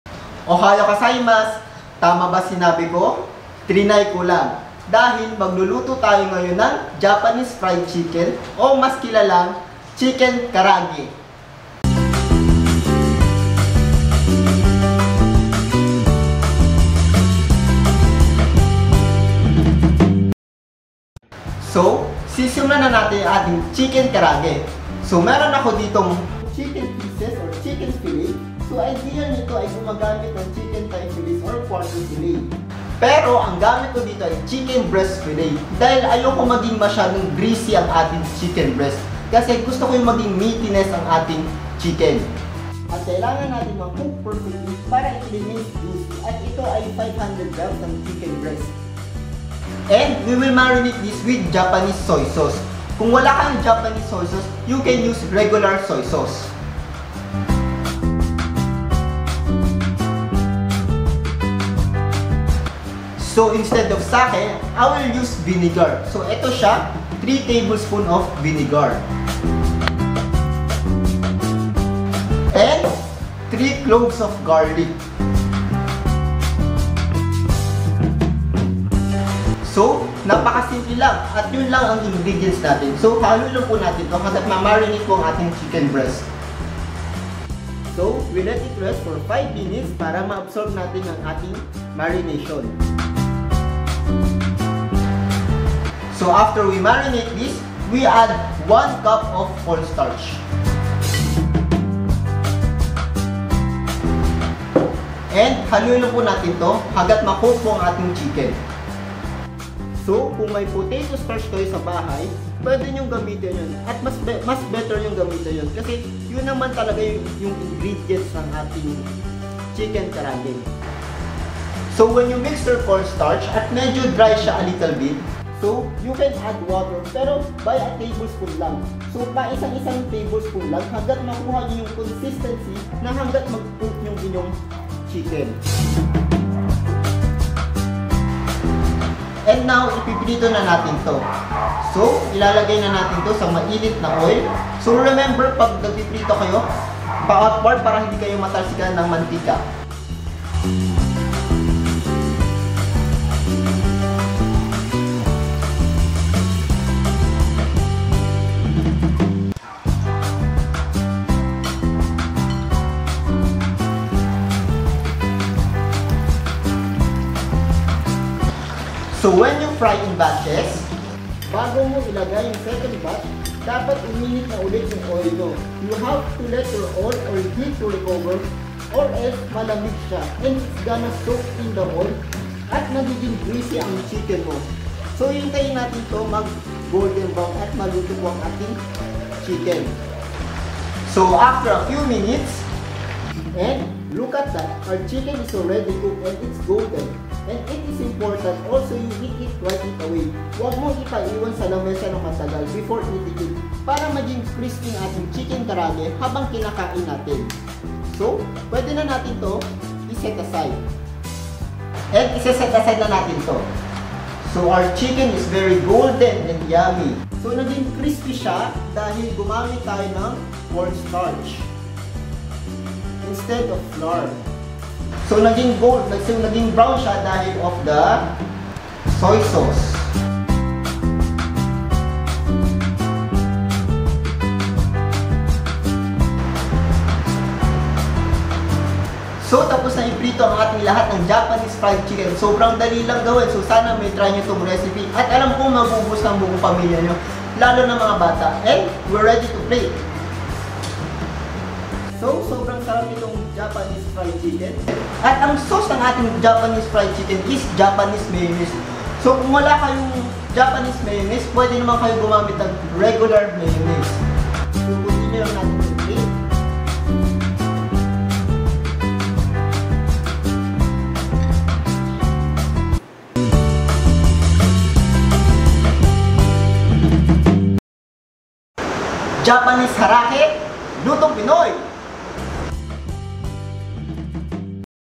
CHICKEN KAARAGE | Chef JB
CHICKEN KARAAGE RECIPE | CHEF JB

We all love fried chicken, and today, we are making our Lutong Pinoy version of our favorite Japanese Fried Chicken. No need to order from the restaurant dahil kaya na nating magluto sa bahay ng very crispy and flavorful Chicken Karaage!🇯🇵

INGREDIENTS:
350 grams chicken breast fillet
3 tablespoons soy sauce
2 cloves minced garlic
1 teaspoon grated ginger
1 teaspoon vinegar
*1 cup cornstarch
1 Nori sheet, cut into strips
cooking oil, for deep frying

To Serve:
Japanese Mayo or regular mayo
Lemon wedges

Chef's Tip:
Double fry the chicken to make it more crisp.

Note:
Add 1-2 tablespoons of water if needed.
*Double the measurement of cornstarch.